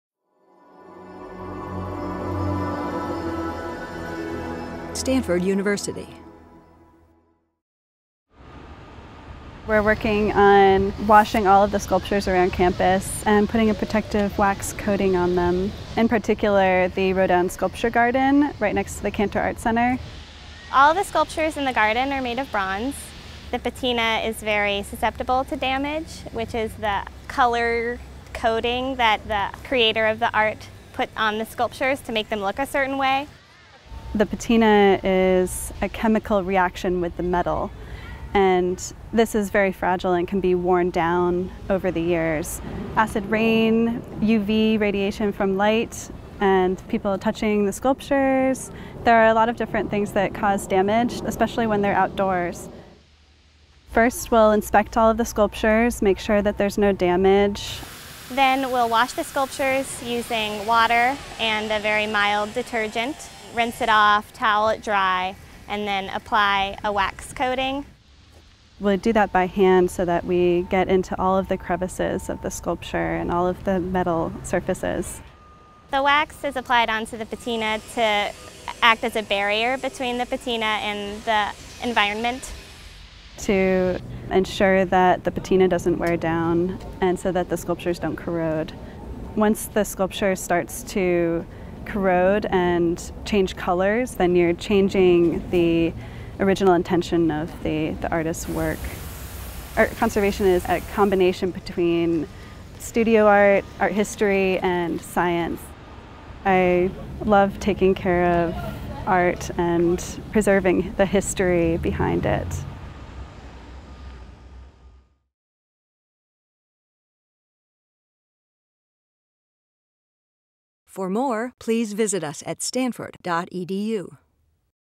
Stanford conservators work to preserve Rodin Sculpture Garden
With dust, UV light radiation and acid rain to contend with, it's difficult to keep outdoor works of art in their original, intended condition. To help fend off corrosion, the Rodin Sculpture Garden, next the Cantor Arts Center, is in the middle of its annual conservation work. Conservators first wash the sculptures, then apply a layer of wax to act as a buffer between the patina and outside elements.